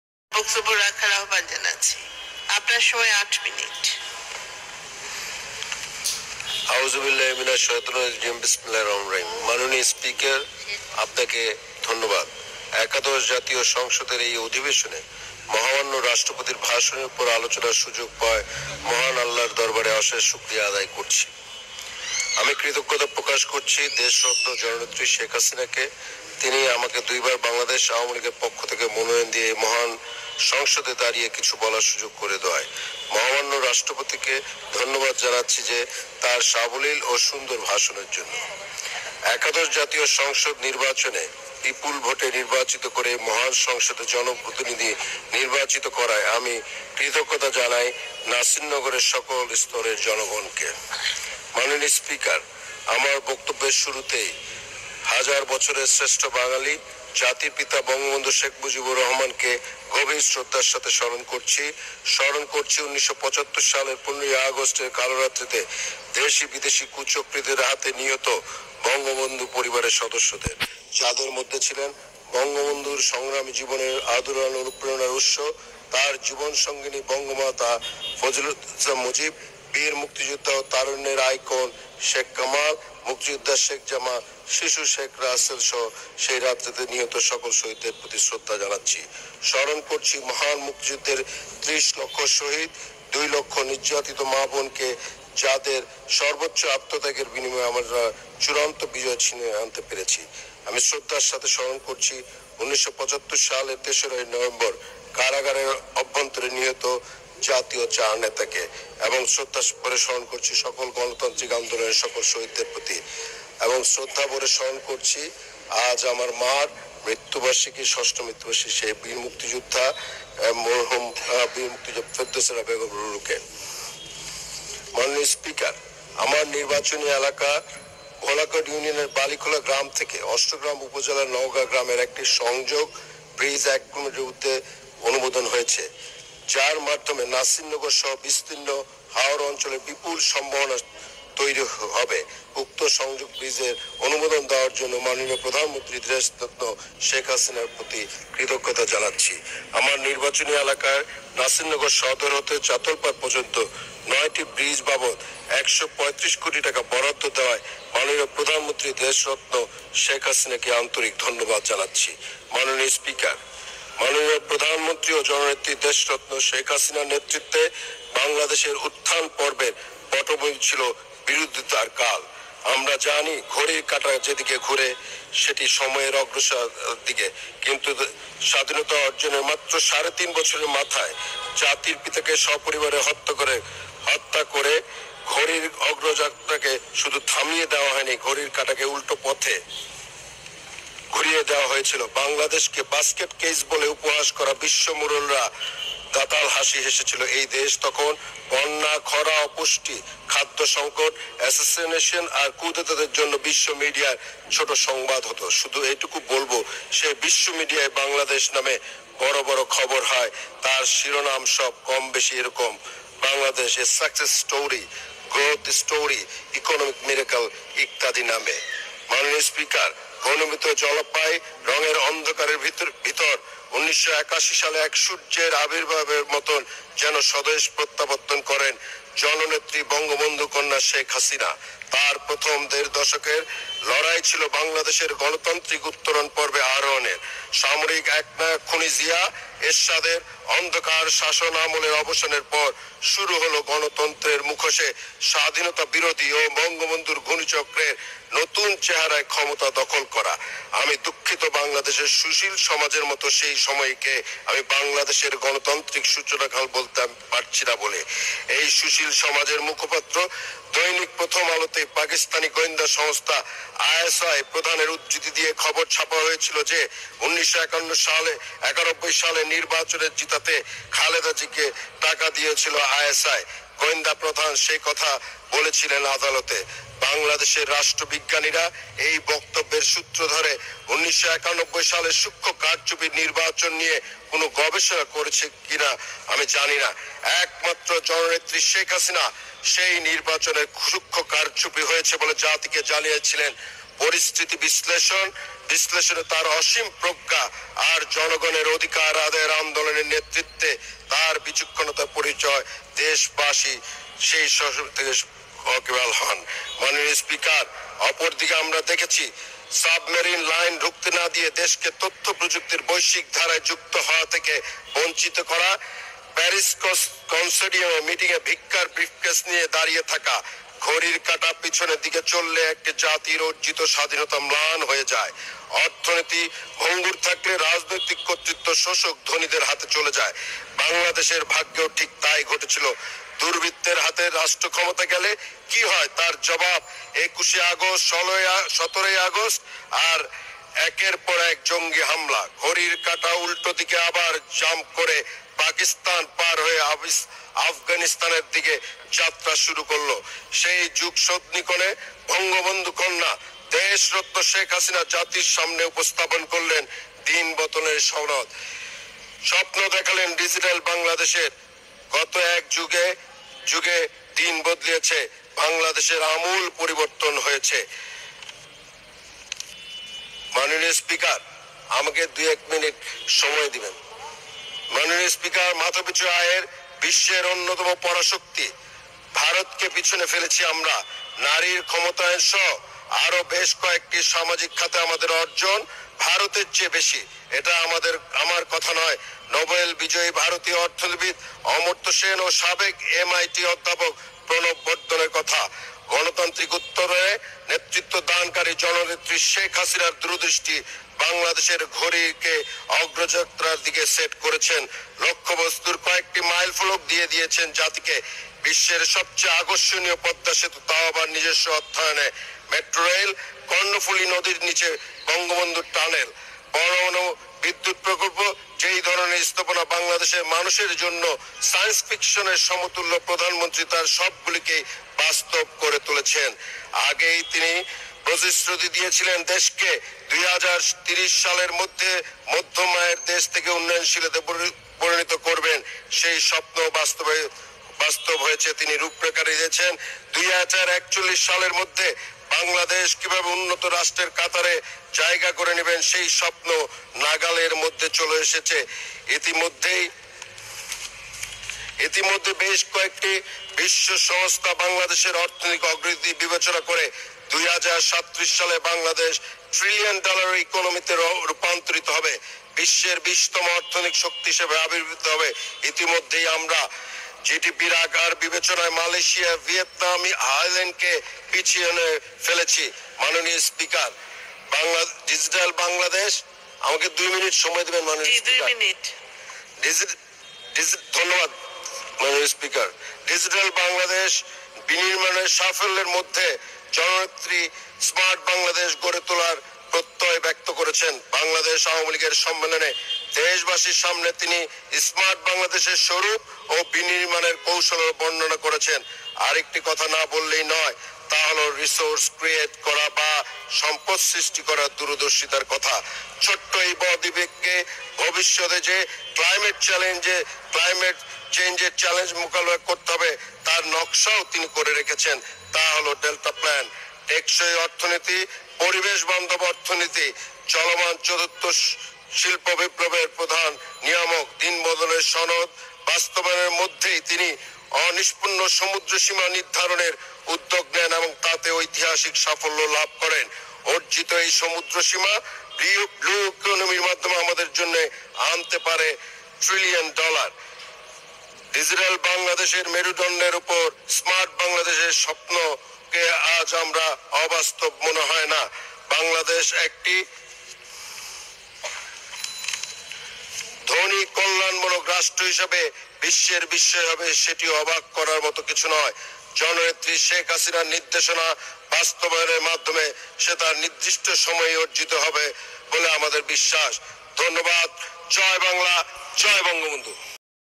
নাসিরনগরের অভাবনীয় উন্নয়নের জন্য প্রধানমন্ত্রী শেখ হাসিনাকে সংগ্রাম এমপির ধন্যবাদ জ্ঞাপন।
বদরুদ্দোজা মো: ফরহাদ হোসেন সংগ্রাম বাংলাদেশের ব্রাহ্মণবাড়িয়া-১ আসনের সংসদ সদস্য। তিনি ২০১৮ সালের ১৩ মার্চ বাংলাদেশ আওয়ামী লীগ থেকে মনোনয়ন নিয়ে উপ-নির্বাচনে সংসদ সদস্য হিসেবে নির্বাচিত হন।
আজ জাতীয় সংসদে মহামান্য রাষ্ট্রপতির ভাষণের উপর বক্তব্য রাখেন তিনি।